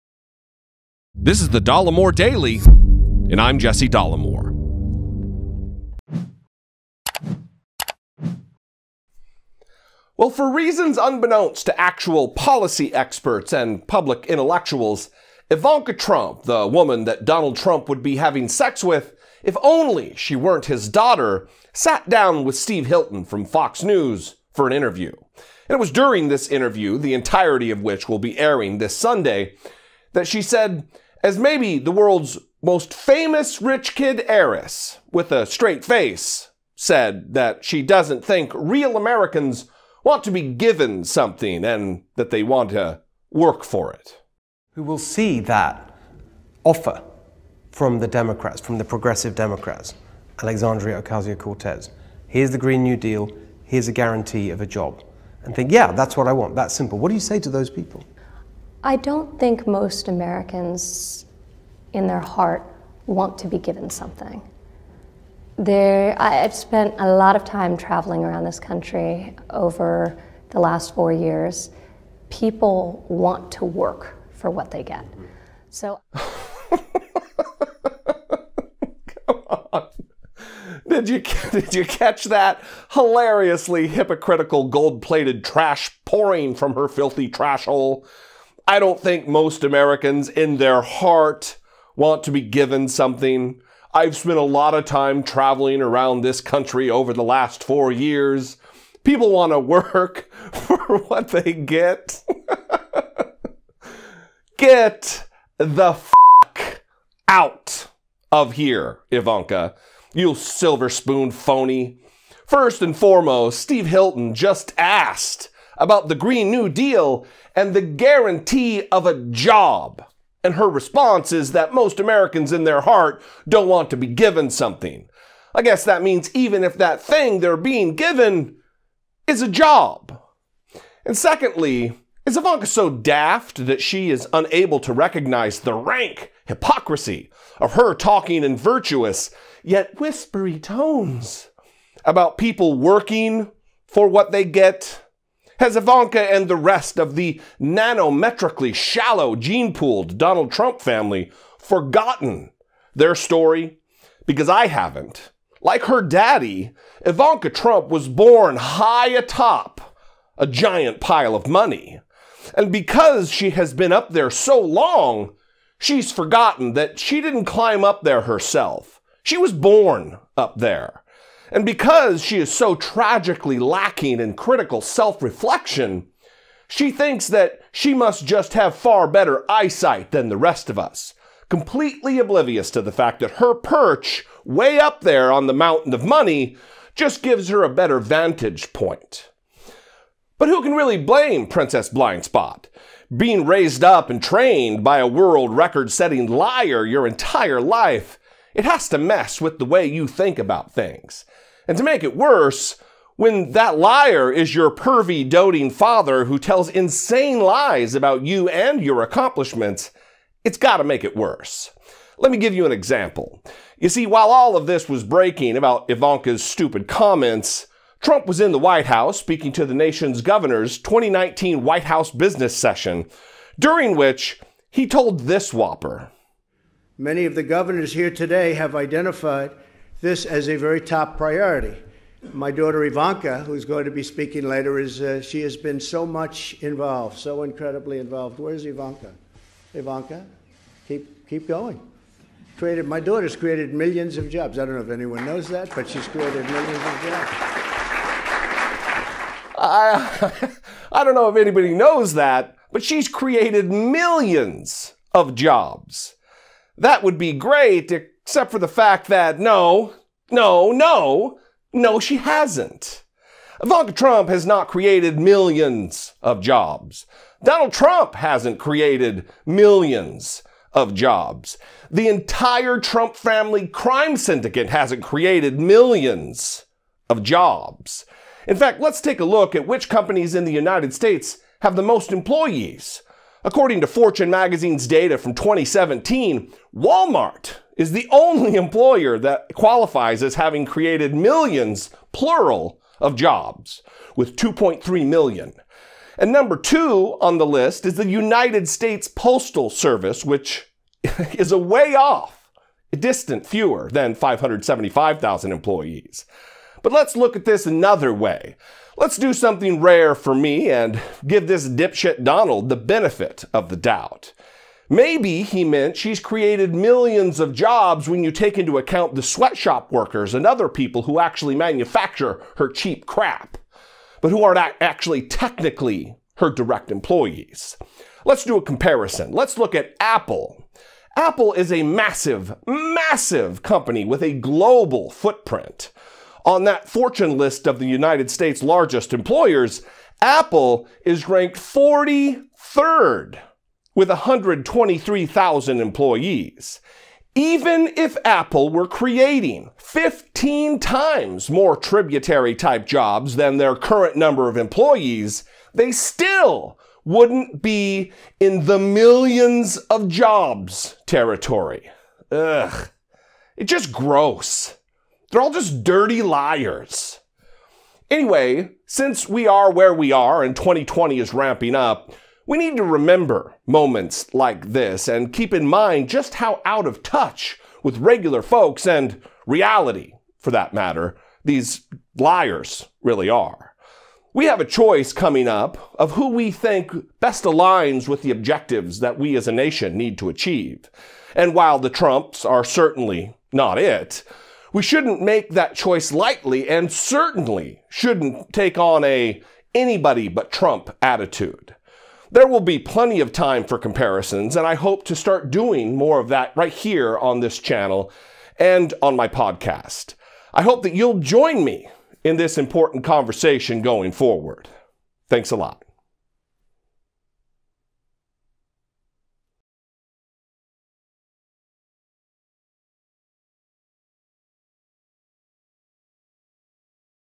HYPOCRITE! Wealthy Heiress Ivanka Trump “Richsplains” Earning to America!
Ivanka Trump told Steve Hilton that, “I don’t think most Americans, in their heart, want to be given something,” - “I’ve spent a lot of time traveling around this country over the last four years. People want to work for what they get." Apparently Ivanka forgot about the fact that she hasn't EARNED anything, and instead was born on a giant pile of money!